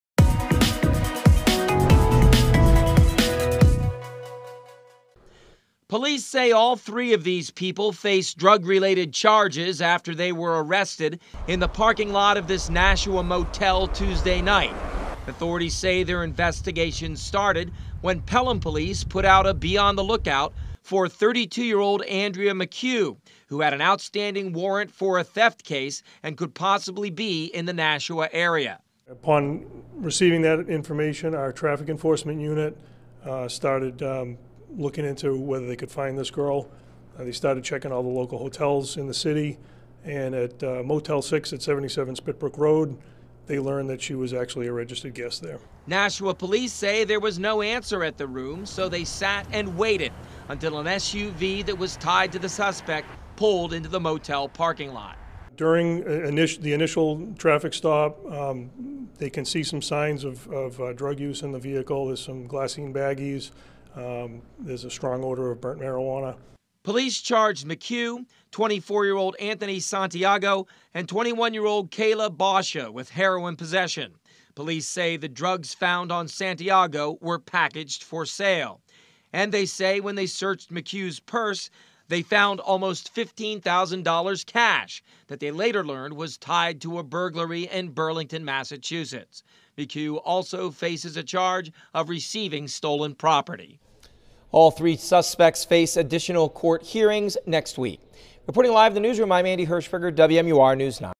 3 face drug-related charges in Nashua
Three people are facing drug-related charges after an arrest outside a Nashua motel.